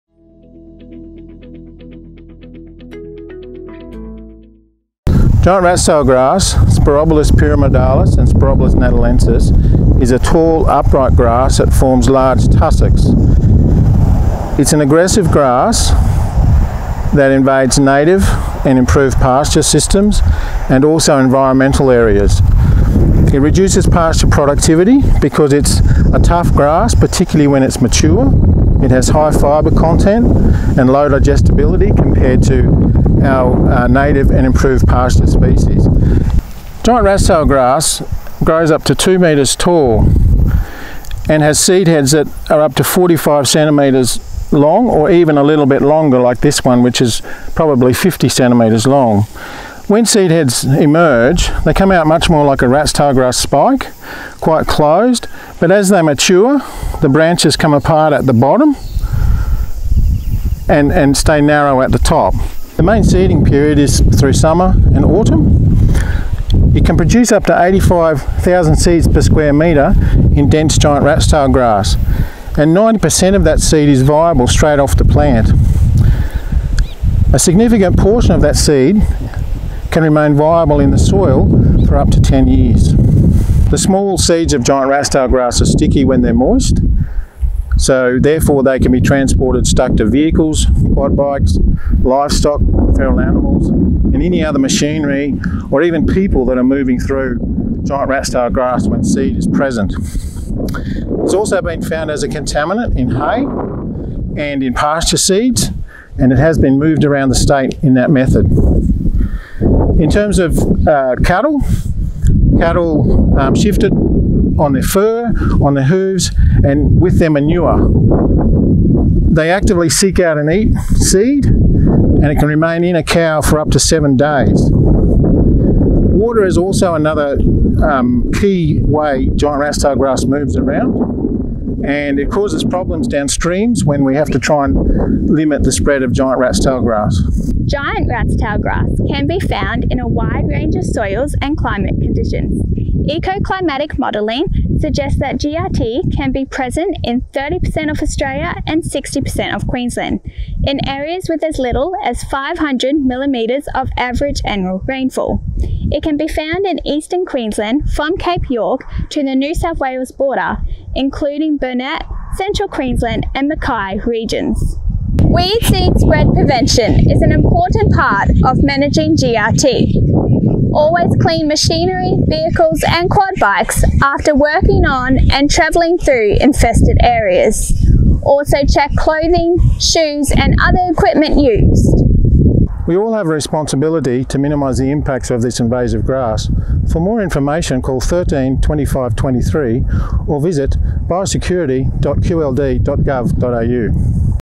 Giant rat’s tail identification, impacts, methods of spread and distribution in Queensland
Giant rat’s tail grass is an aggressive grass that can reduce pasture productivity, out-compete desirable pasture grasses and cause significant degradation of natural areas. It can outcompete, and in some cases, totally eliminate native flora.